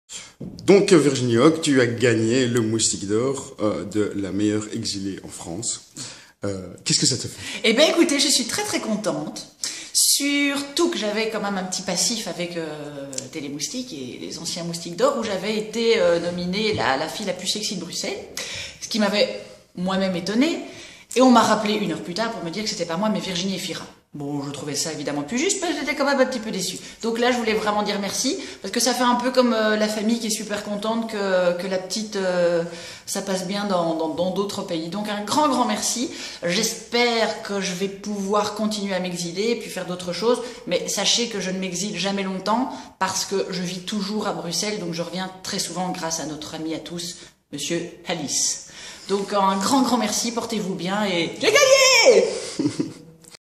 Réaction de Virginie Hocq suite à son Moustique d'Or
Virginie Hocq a été désignée Moustique d'or de la meilleure Belge en exil. Réaction à chaud